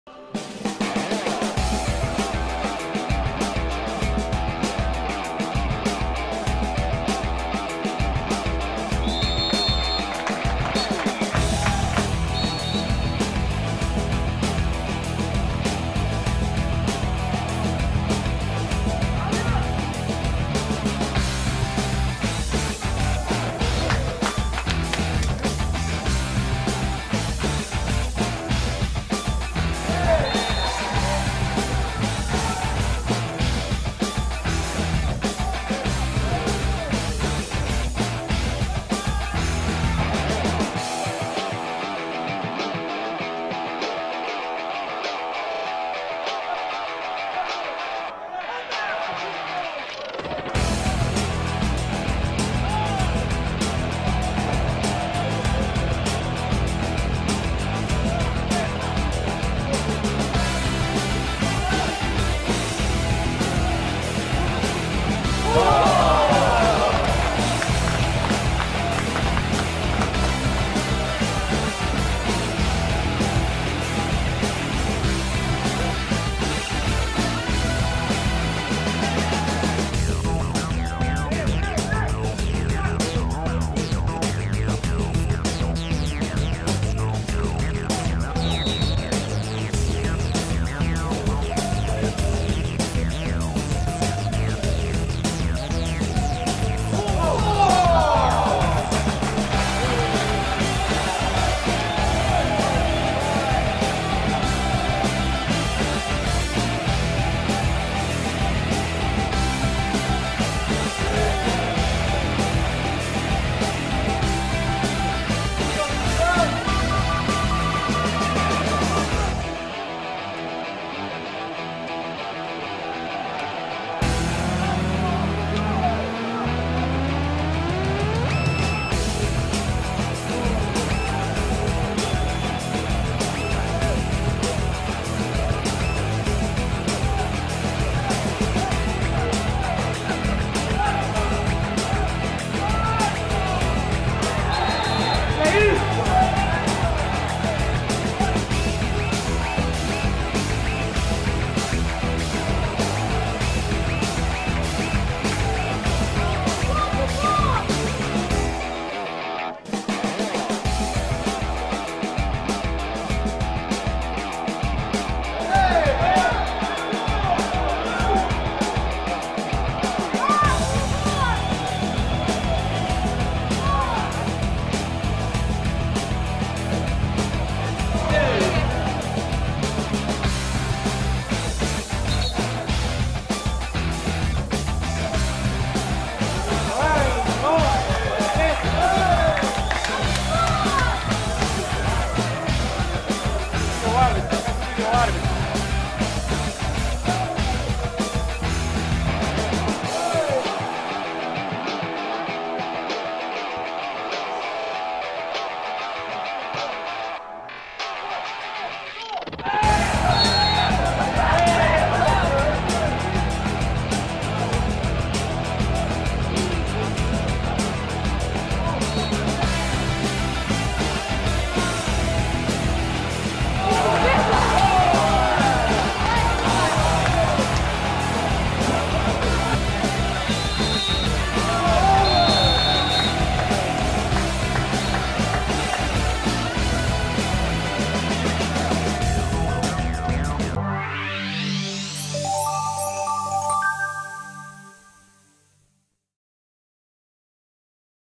FC Alverca vs Alta de Lisboa - Resumo do Jogo
Resumo do jogo FC Alverca vs Alta de Lisvoa, realizado a 21 de Maio de 2017 e que contou para a 30ª jornada do Campeonato Pro-Nacional 2016/17, da Associação de Futebol de Lisboa.